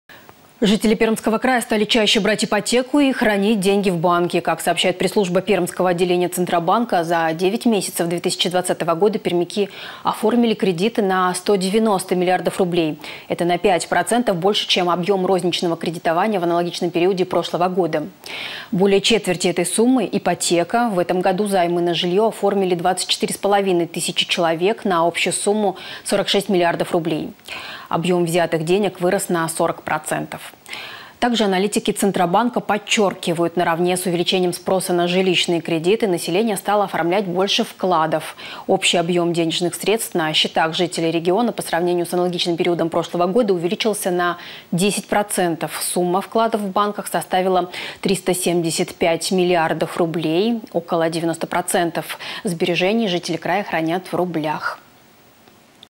Пермяки стали чаще брать ипотеку и хранить деньги в банке
Жители Пермского края стали чаще брать ипотеку и хранить деньги в банке. По данным  Пермского отделения Центробанка, за девять месяцев текущего года пермяки оформили кредиты на 190 млрд рублей. Более четверти этой суммы – это ипотечные займы. Также чаще пермяки стали открывать вклады. Общий объем денег на счетах жителей Пермского края за год увеличился на 10%. Сумма вкладов в банках составила 375  млрд рублей.